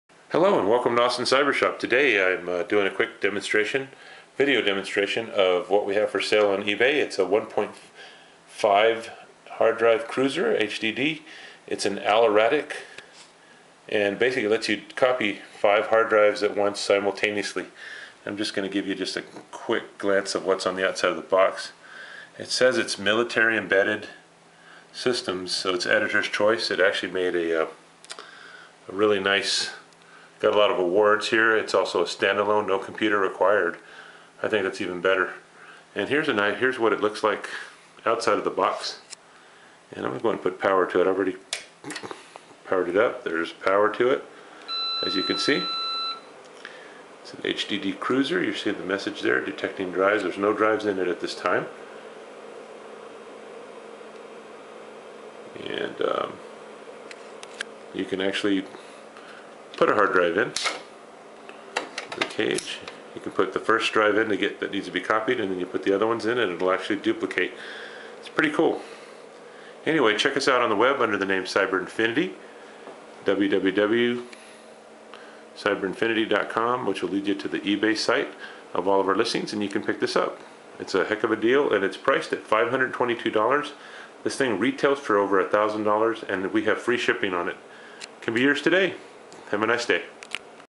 SATA HARD DISK ERASER AND COPIER, ALERATEC 350104, 1:5, Duplicator, Sanitizer at cyberinfinity
Here's a video for one of our SATA HARD DISK ERASER AND COPIER, ALERATEC 350104, 1:5, Duplicator, Sanitizer, enjoy!
If you have a bunch of sata hard drives that need to be erased and cleaned off in batches of 5 at a time, this is what you need. It can also copy and duplicate the same info on 5 sata drives at a time.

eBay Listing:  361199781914